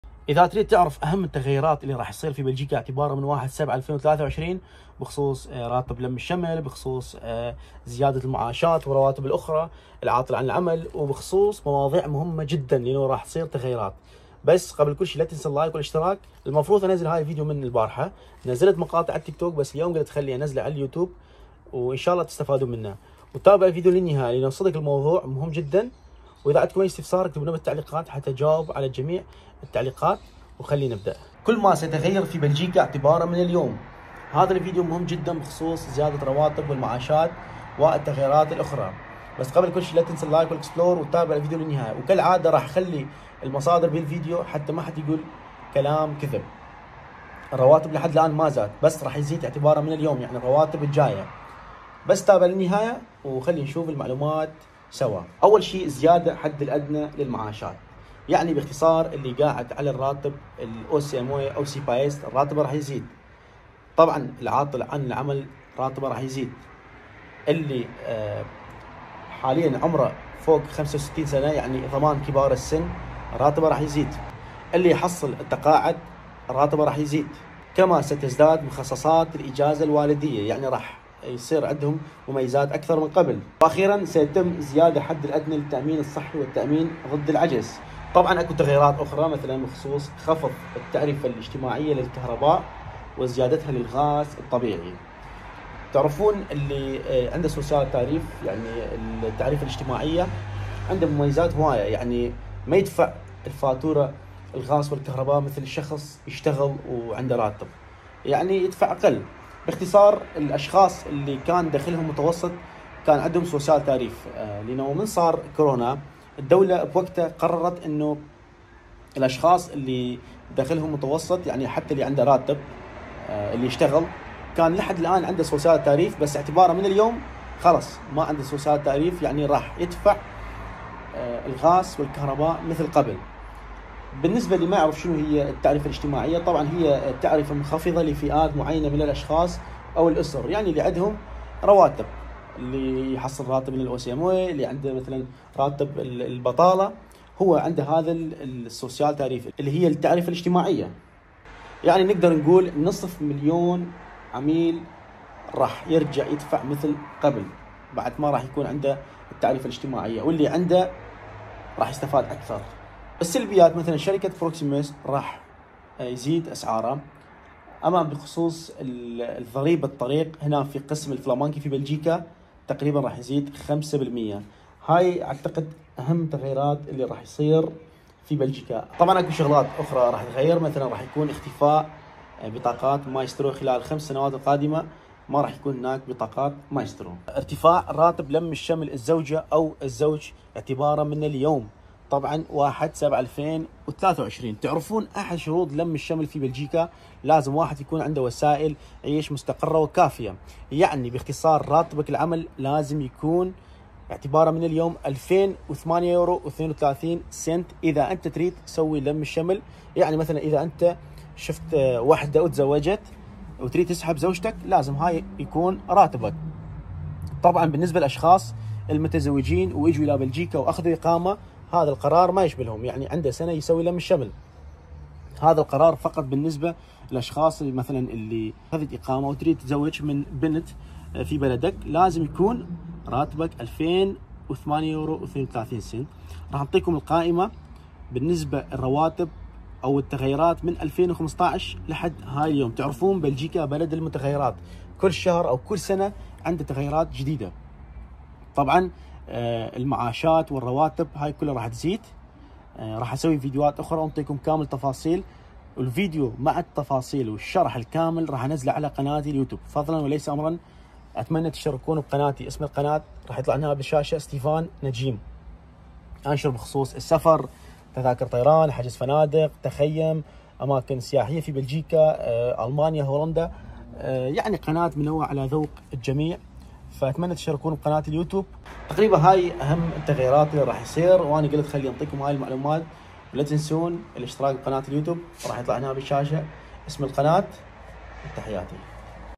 كلما سيتغير في بلجيكا اعتبارا من 1/7/2023 | لم الشمل، زياده رواتب و تفاصيل مهمه
كلما سيتغير في بلجيكا اعتبارا من بدايه شهر السابع | زياده رواتب و المعاشات و راتب لم الشمل و مساعدات من الحكومه و تفاصيل مهمه